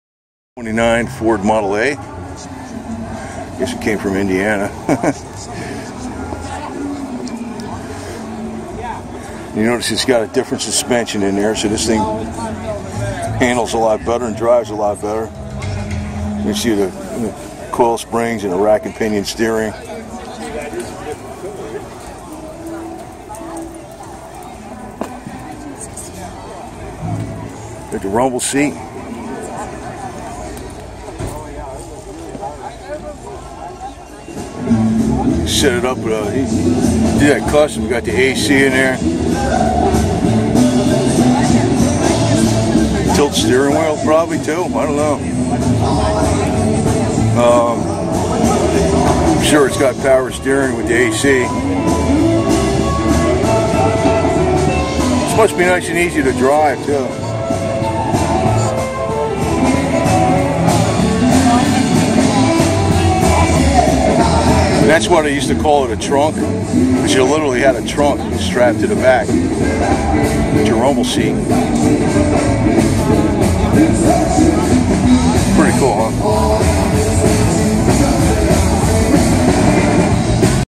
1929 Ford Model A
1929 Ford Model A
Dec 2022, the Villages Florida car show, muscle cars, vintage cars, classic cars, cool rides